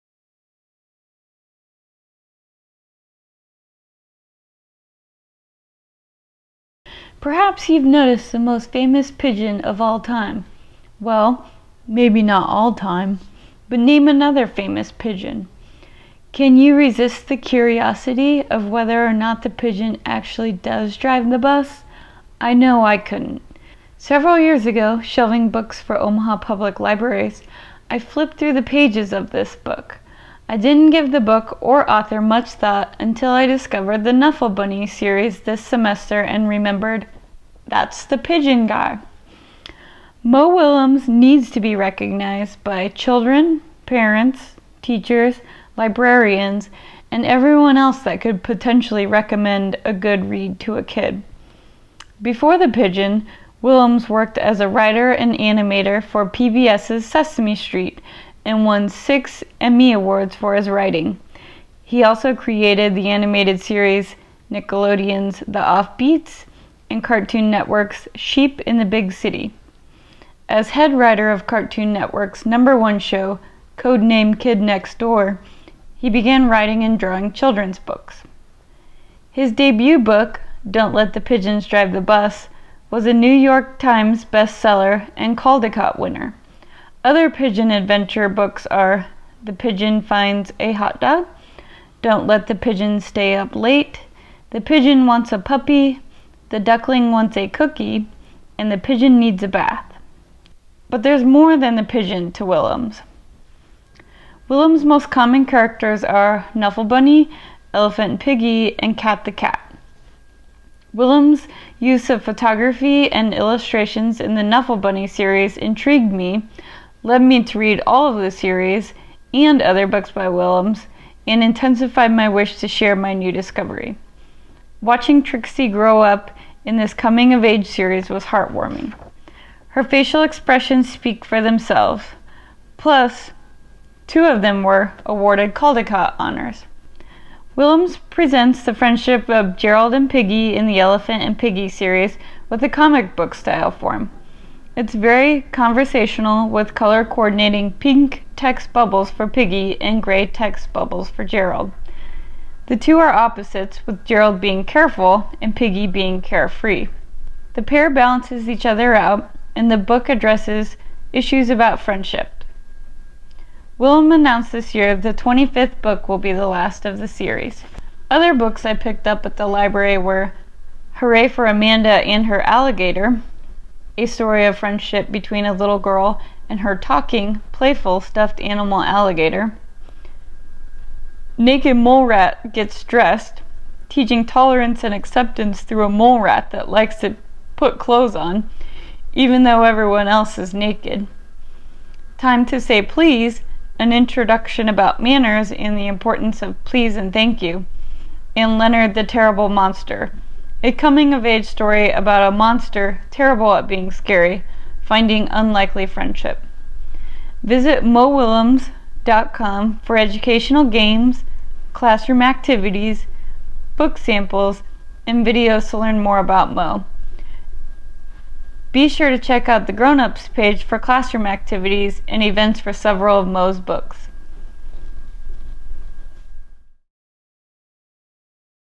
Meet Mo - Mo Willems Author Profile
Mo Willems Author Profile video made for Children's Literature class.